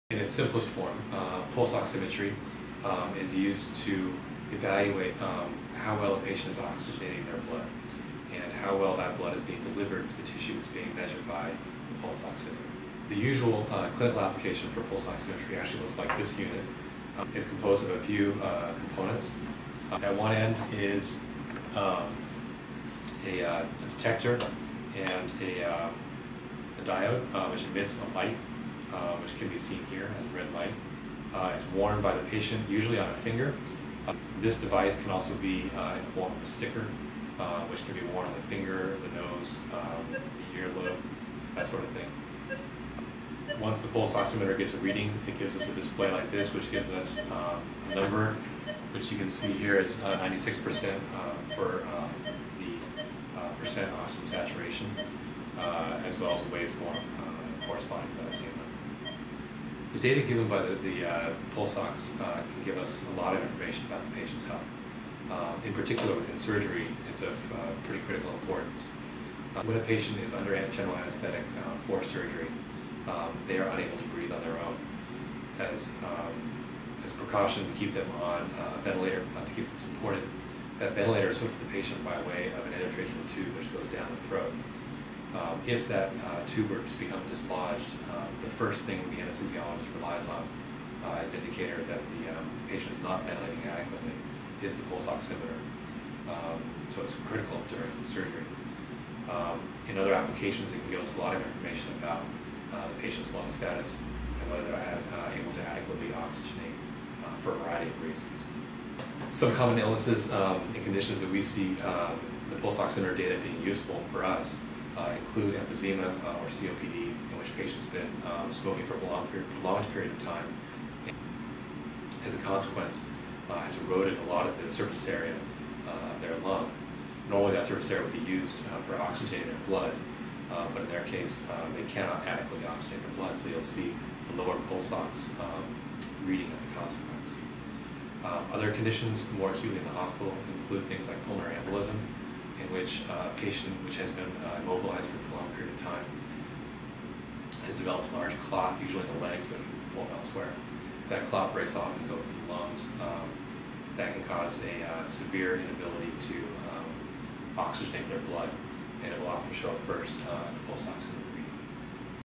Pulse Oximetry - Introduction (Part 2 of 5)
Dr. Bronson describes the basic components of a pulse oximeter and explains how it is used to monitor a patient’s blood oxygen saturation levels to assist doctors and nurses in monitoring a patient’s condition.